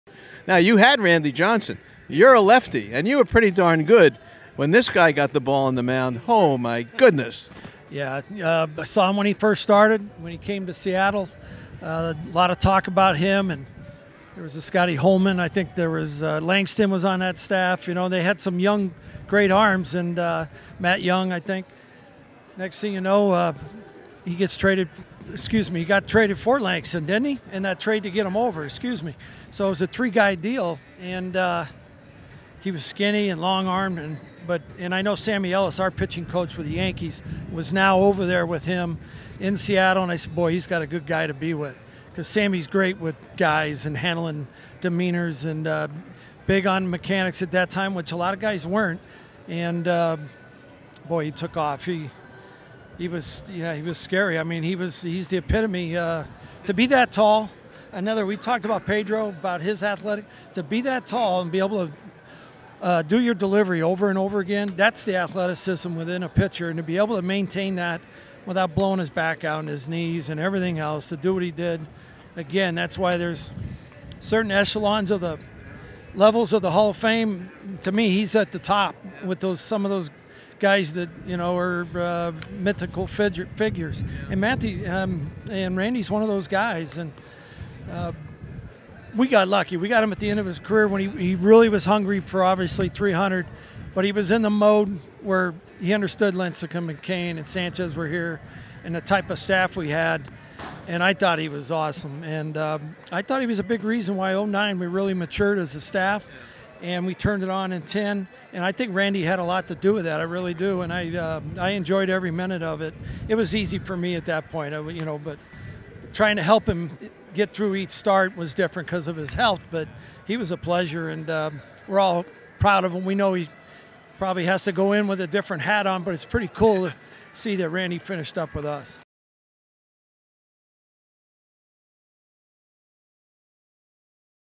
Righetti: Hall of Famer Randy Johnson set up Giants' 2010 WS win
Giants pitching coach Dave Righetti spoke with KNBR 680's Marty Lurie about former Giants pitcher Randy Johnson and his upcoming Baseball Hall of Fame induction.